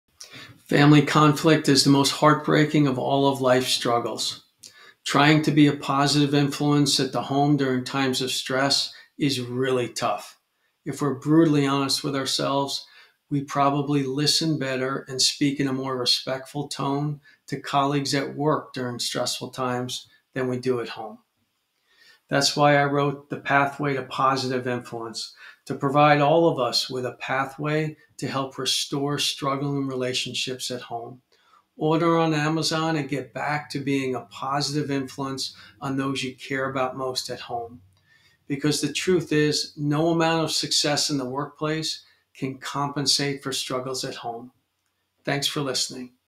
How to have a positive influence during stressful events at home
The Pathway to Positive Influence can provide a new path to help restore struggling relationship that are most important to us in the home. The path starts with the other person you care about and opening a door to their story…It is about THEM, not US.